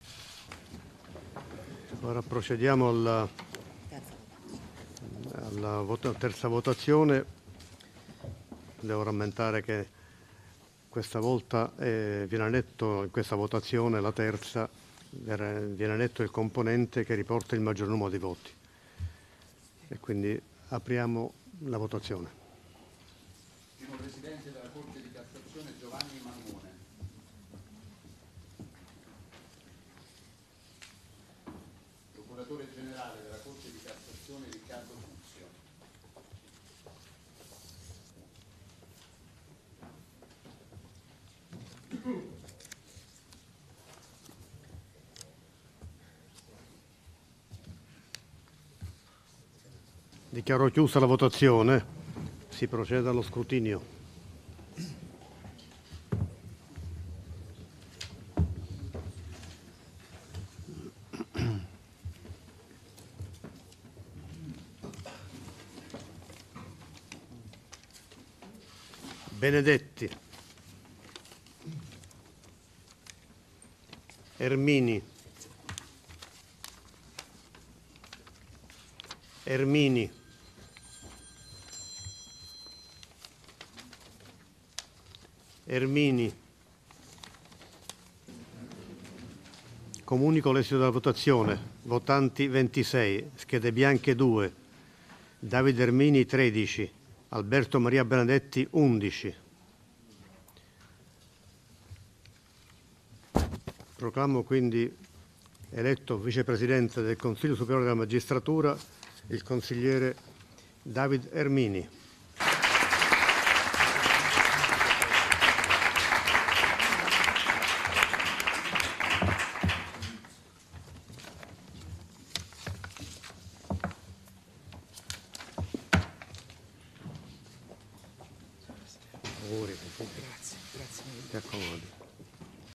Ermini eletto Vice Presidente del Csm
(Agenzia Vista) - Roma, 27 Settembre 2018 - Ermini eletto Vice Presidente del Csm.